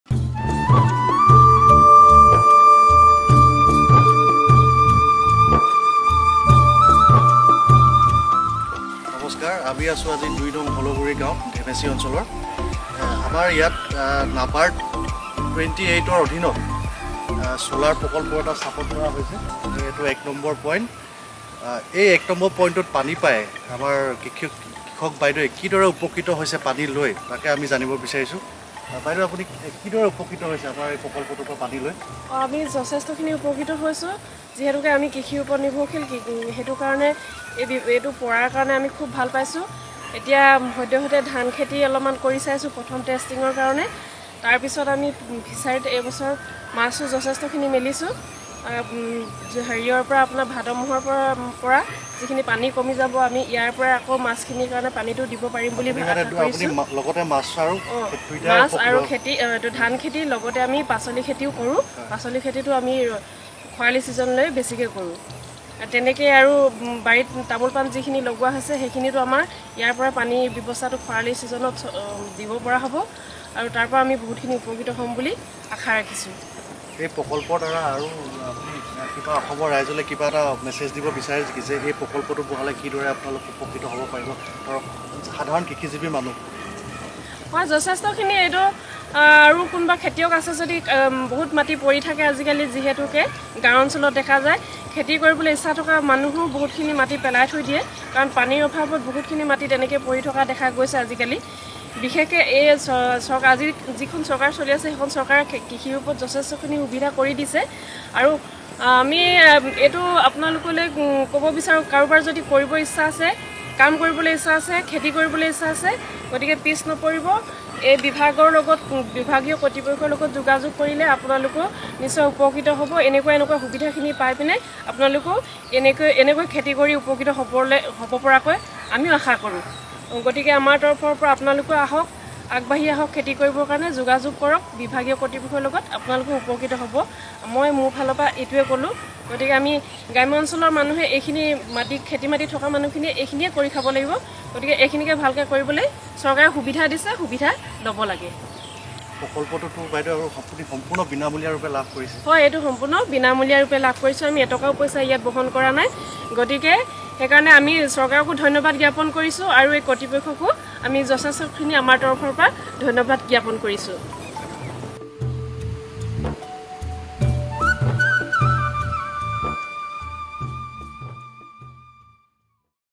Moran - Tingkhong division
অবিৰত জলসিঞ্চন, সমৃদ্ধ খেতিপথাৰ!

মৰাণ-টিংখং সংমণ্ডলৰ দ্বাৰা ডিমৌ কিনাৰ গোঁহাই গাঁৱত RIDFXXVIII-২০২২-২৩ বৰ্ষৰ অধীনত ৰূপায়িত মধ্যম গভীৰ নলী-নাদ আঁচনিৰ ৩ টা পইন্টৰ জৰিয়তে কৃষিভূমিলৈ অবিৰতভাৱে পানী প্ৰদান কৰি থকা হৈছে।

আঁচনিখনৰদ্বাৰা লাভান্বিত কৃষকে নিজৰ মনৰ ভাৱ ব্যক্ত কৰে এনেদৰেঃ